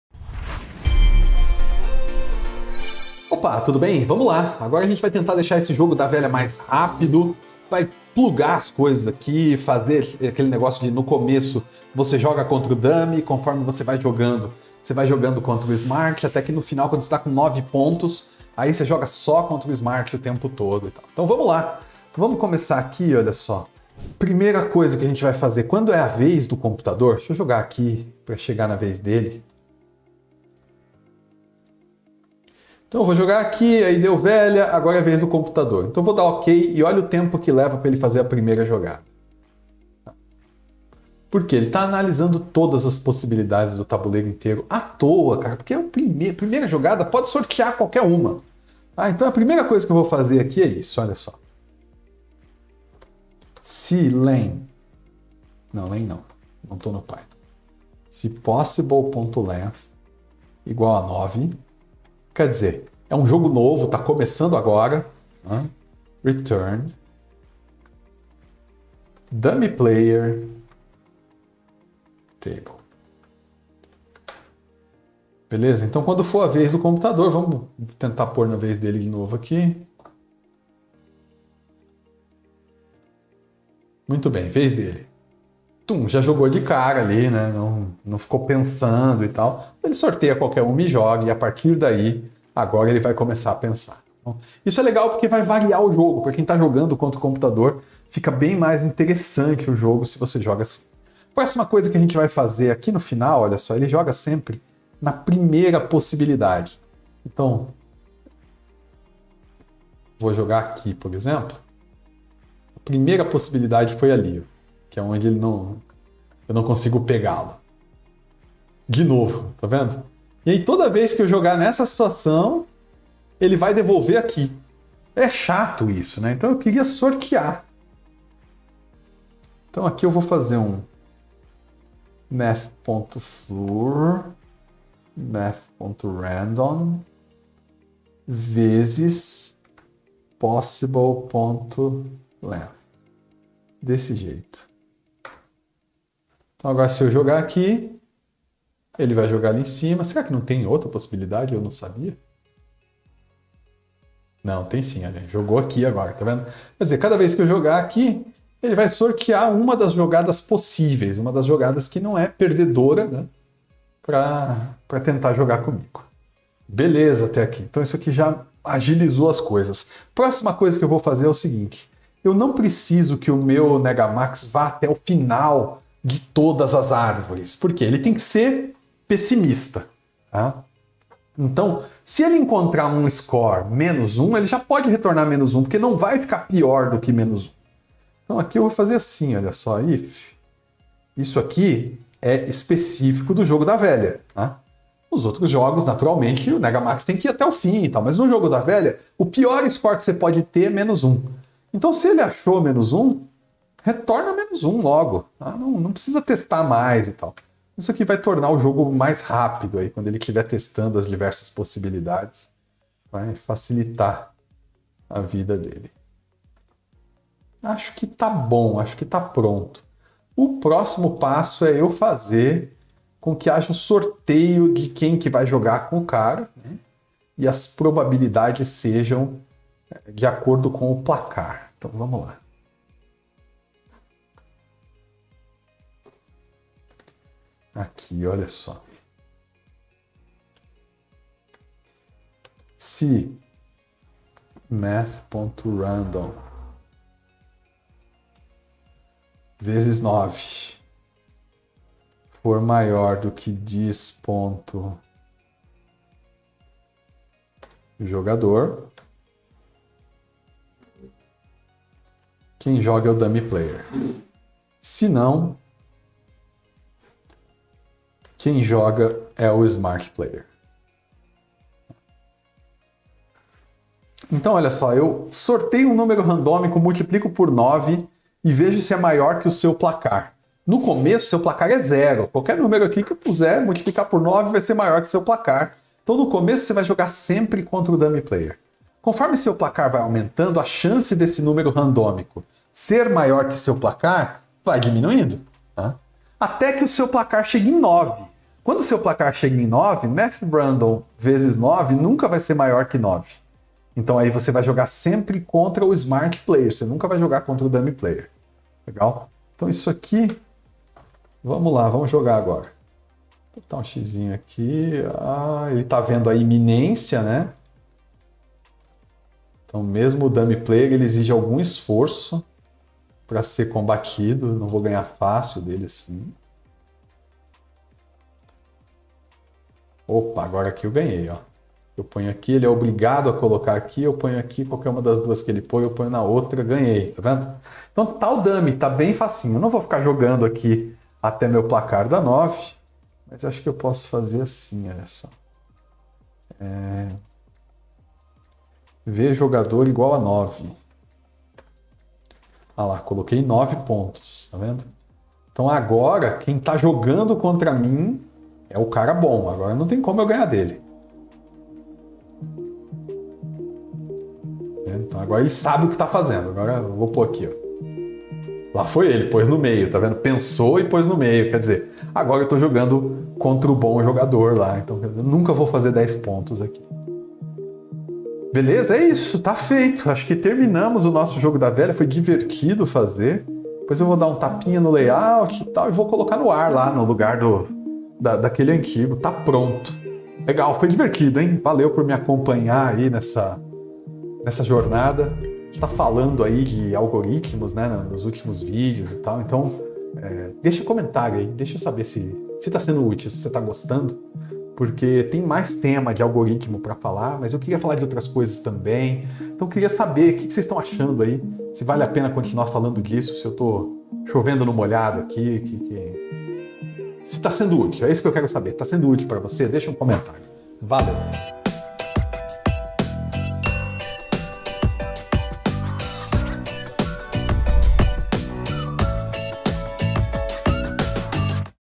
Jogo da Velha 4/4: otimizando o algoritmo
Neste último vídeo da série do jogo da velha, vamos otimizar o jogo e colocar tudo no lugar, deixando pronto para publicação.